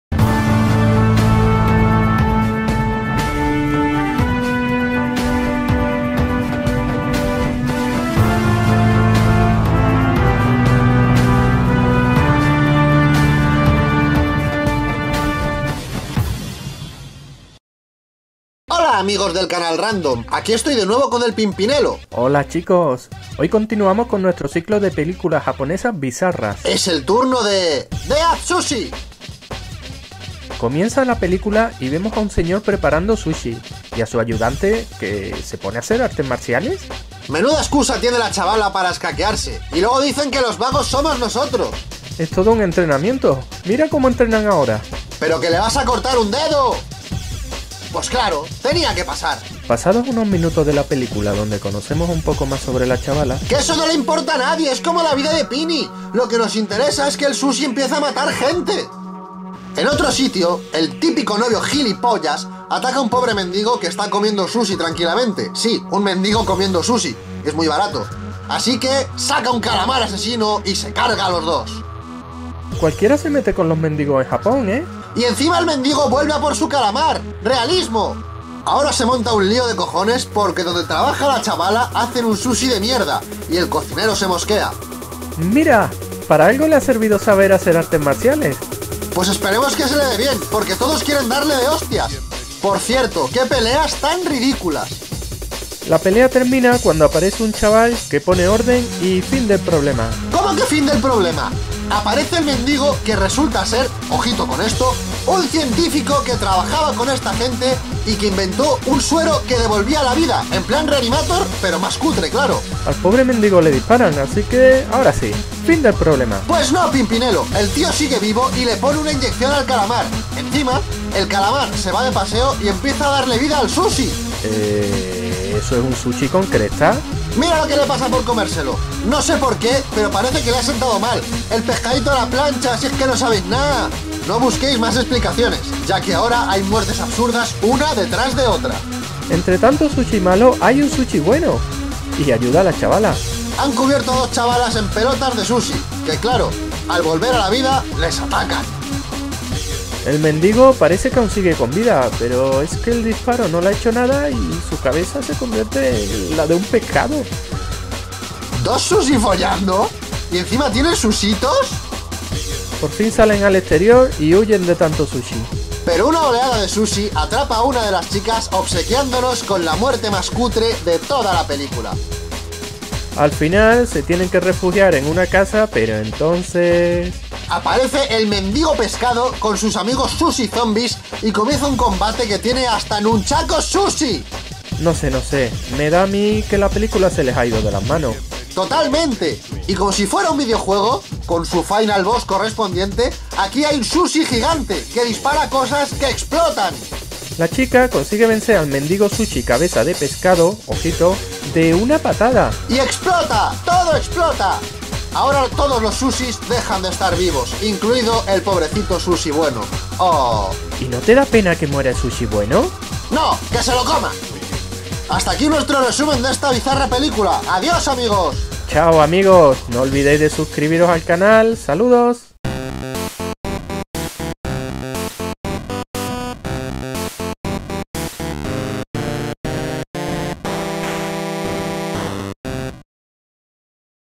Películas Japonesas Gore Bizarras: Dead Sushi REVIEW y Crítica
Nuevo vídeo dedicado a nuestro ciclo dedicado a las películas japonesas bizarras, esta vez con Dead Sushi. Comentado por Thrawn y Pimpinelo.